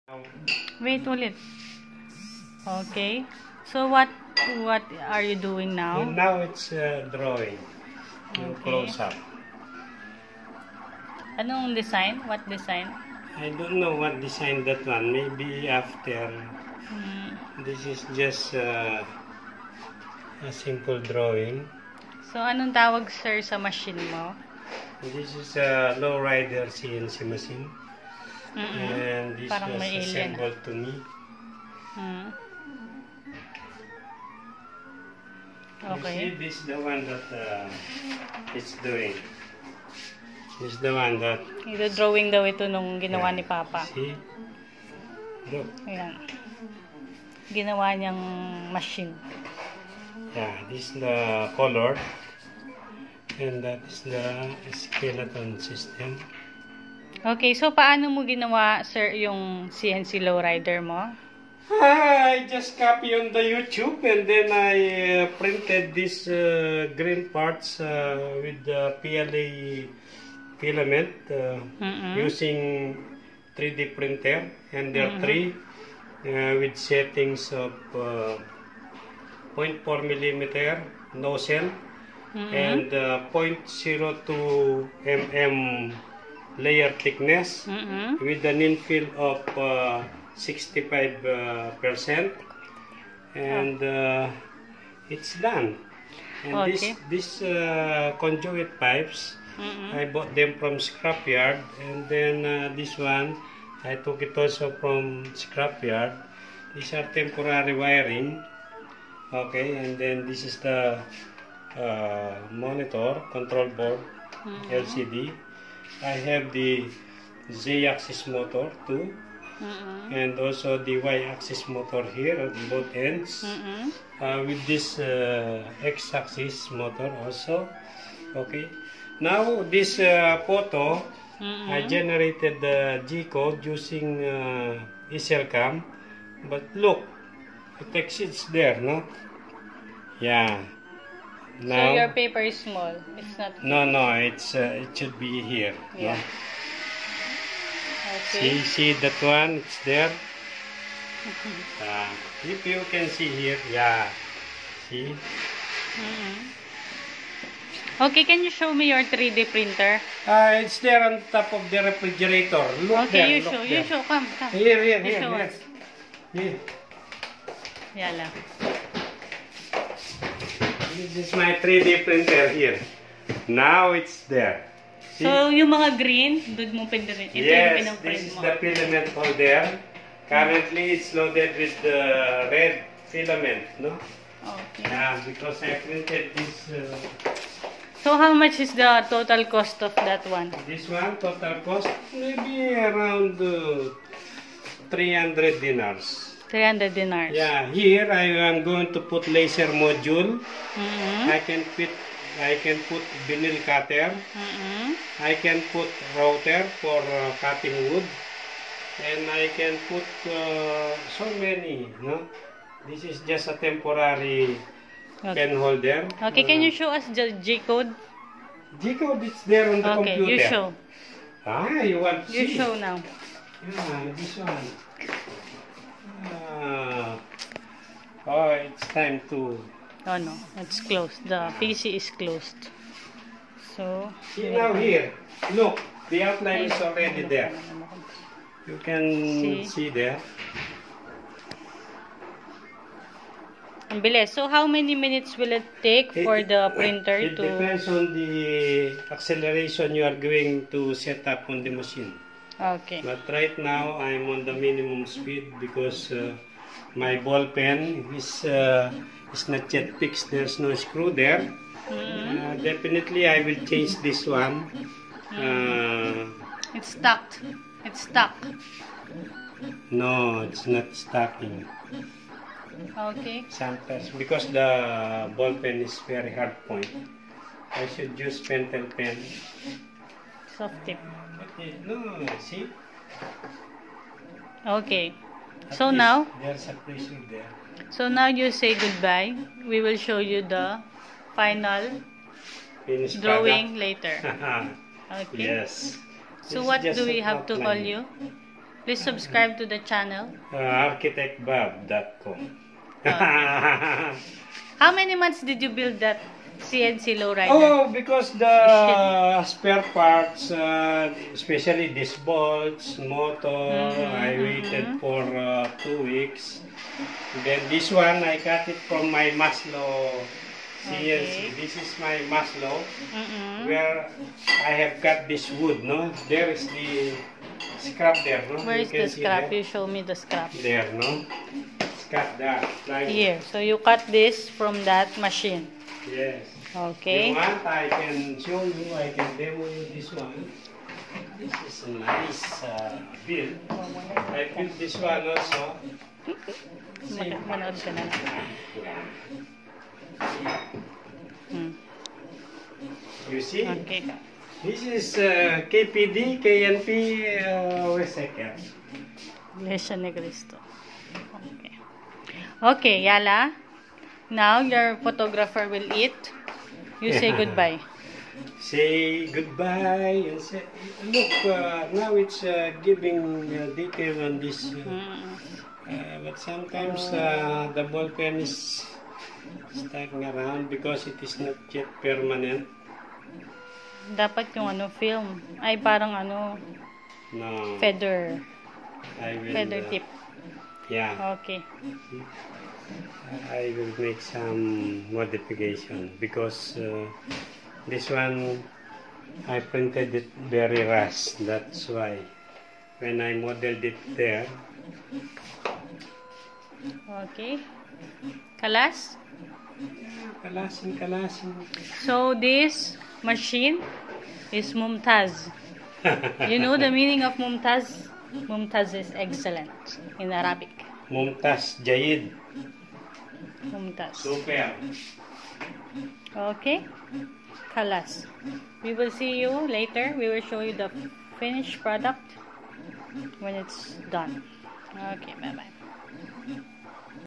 LowRider 2 CNC with ballpen mount
plotting family picture in Bahrain